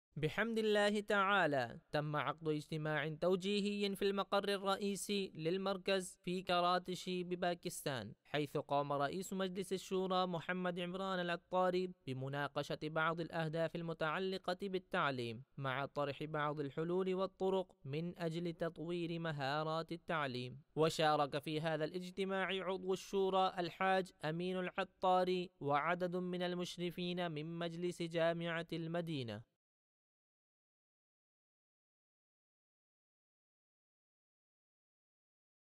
التقرير الأخباري || اجتماع توجيهي بين أعضاء مجلس جامعة المدينة
يهتم مركز الدعوة الإسلامية بتوجيه الموظفين والحرص على متابعة أعمالهم من خلال عقد الاجتماعات التوجيهية وتقديم حلول وما شابه ذلك
...لمعرفة التفاصيل شاهد الفيديو

●▬▬▬▬▬▬▬▬▬ஜ۩۞۩ஜ▬▬▬▬▬▬▬▬▬●